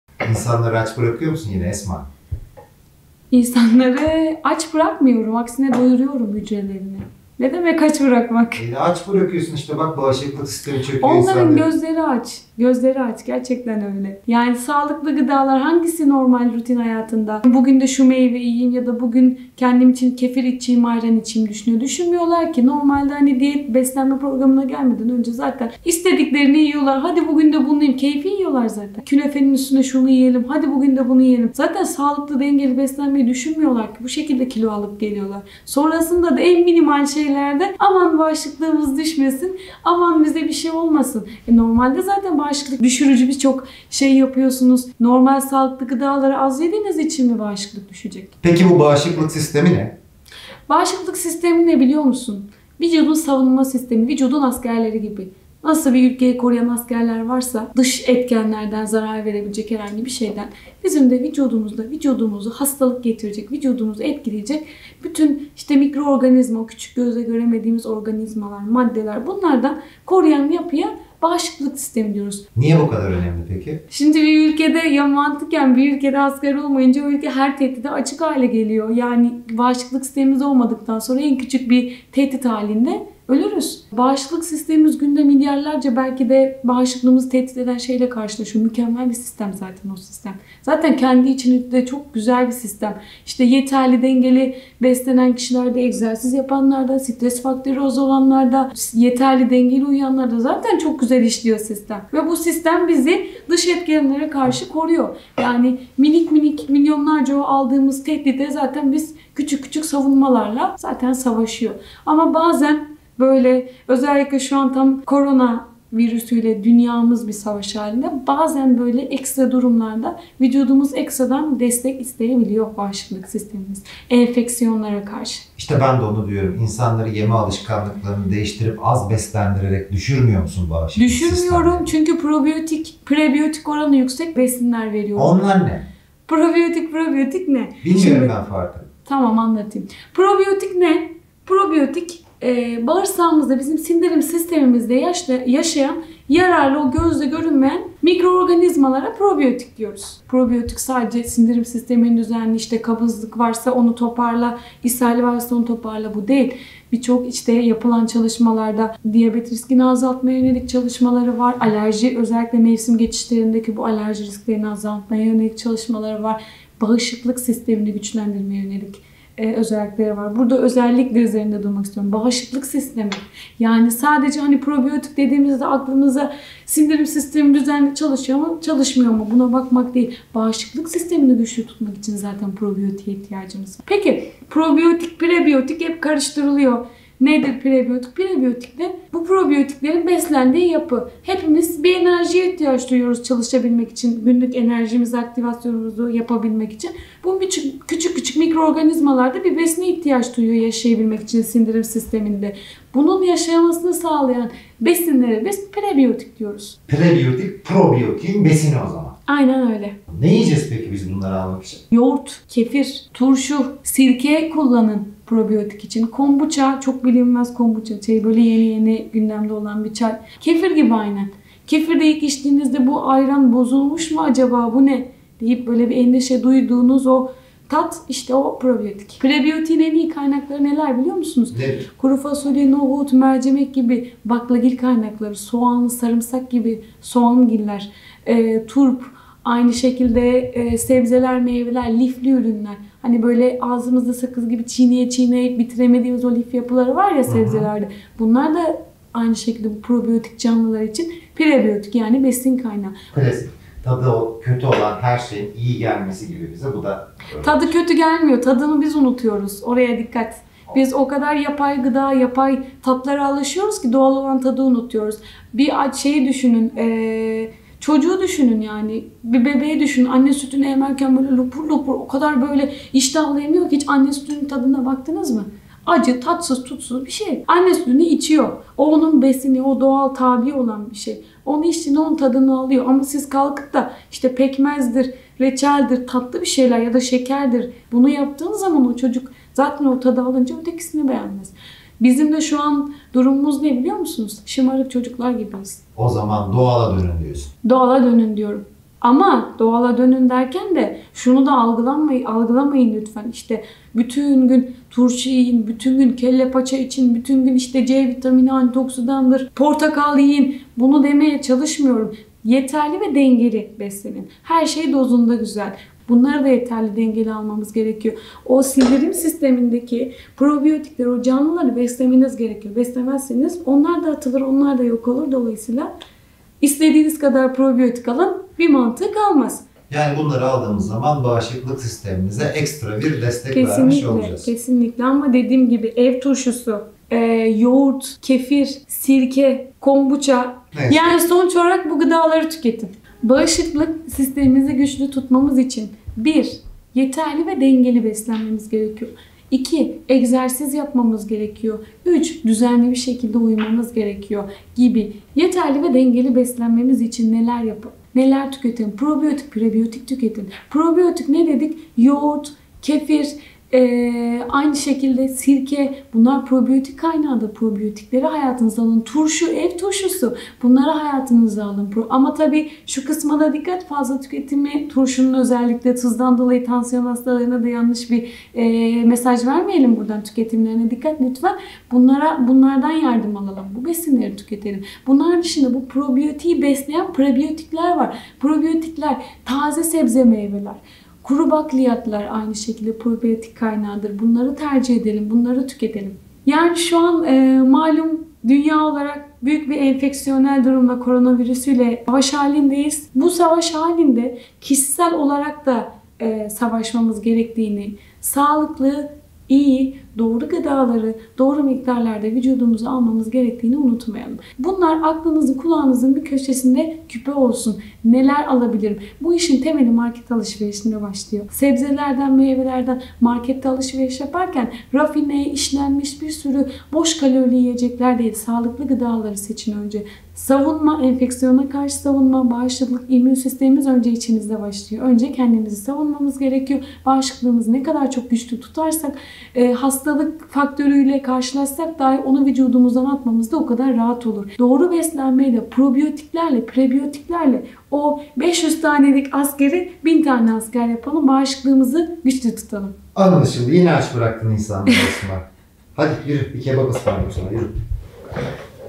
EvdeKal Prebiyotik ve Probiyotik nedir?
Karantina günlerinde nasıl beslenmeliyiz? Bağışıklık sistemimizi güçlü tutabilmek için hangi besinleri tüketmeliyiz. Probiyotik ve Prebiyotik arasındaki fark nedir? Hangi besinler Probiyotik ve Prebiyotik barındırır. Bu besinleri tüketmenin insan sağlığındaki önemi nedir? Sağlıklı beslenmek için aç kalmalı mıyız? Diyet yapmak bağışıklık sistemini zayıflatır mı? Daha kuvvetli bir bağışıklık sistemi için neler yapmalıyız? Tüm bu soruları Dyt. Esma Özdemir ile konuştuk.